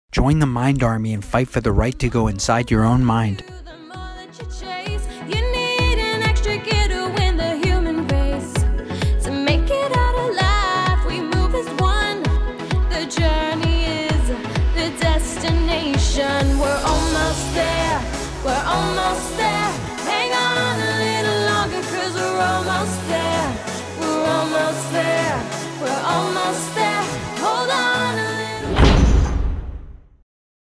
Psychedelics PSA | "It's Your Mind, Can You Hack It?" | Mind Army | Zappy | Qil | Andreas Jimenez
First Ever Public Service Announcement Supporting Psychedelics. The 'Mind Army' wants you to fight for the right to go inside your own mind!

Try this simple bio-hack. Go inside your own mind using meditation, breathing, plant medicines / psychedelics and other useful techniques. Check this channel for more 'Mind Army' PSA's, videos, music and more.

Join the 'Mind Army' and fight for the right to go inside your own mind.

This video was shot on location in Memphis, Tennessee and Kuttawa, Kentucky in August 2017.

Video directed by Zappy Zapolin
Video production by Andreas Jimenez